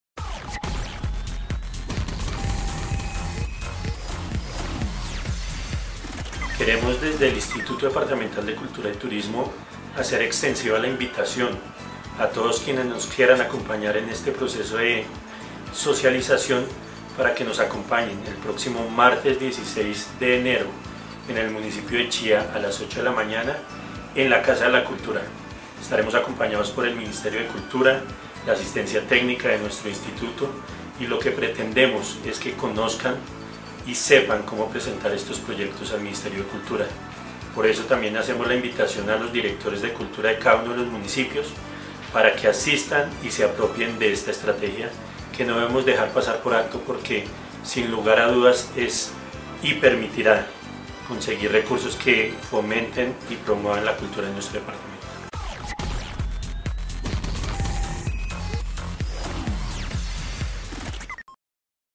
Invitación Idecut Jornada de Socialización Estímulos 2018 - Chía, Cund.
El Gerente del Instituto Departamental de Cultura y Turismo, IDECUT, extiende invitación a participar de la Jornada de Socialización del Programa Nacional de Estímulos 2018 del Ministerio de Cultura Mincultura a realizarse el martes 16 de enero en el Municipio de Chía, Casa de la Cultura, Salón Suatí a partir de las 8:30am